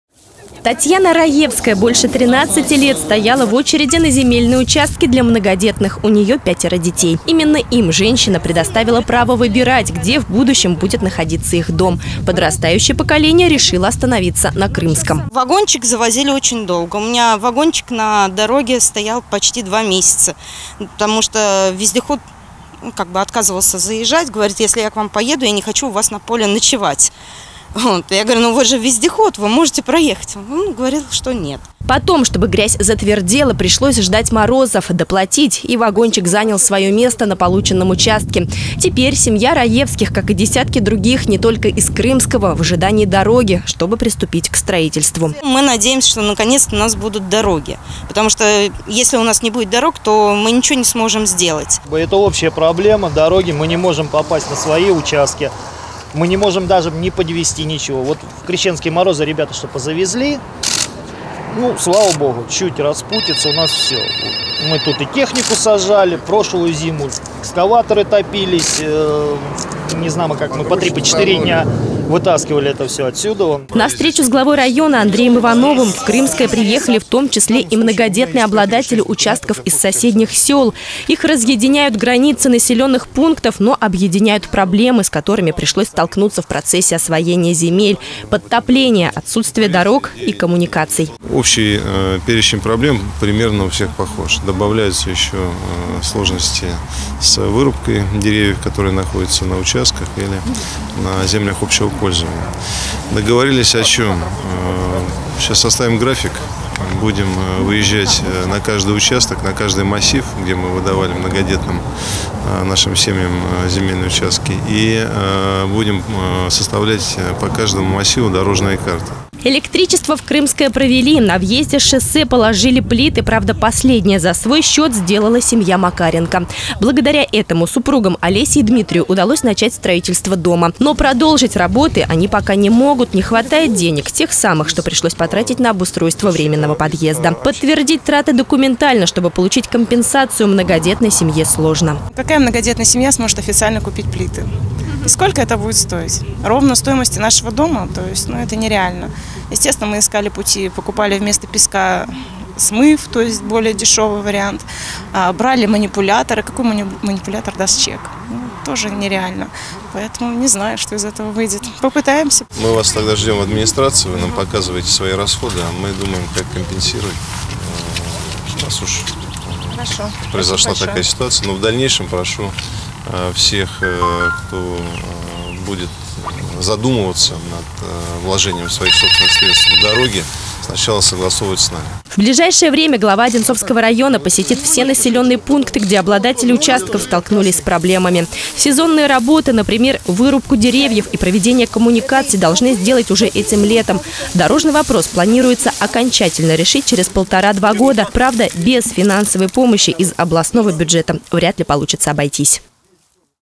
Андрей Иванов встретился с многодетными семьями района
Более 650 участков получили многодетные семьи Одинцовского района. Радость счастливым обладателям земли не омрачило даже то, что некоторые наделы оказались подтопленными, а подъездные пути и коммуникации отсутствовали.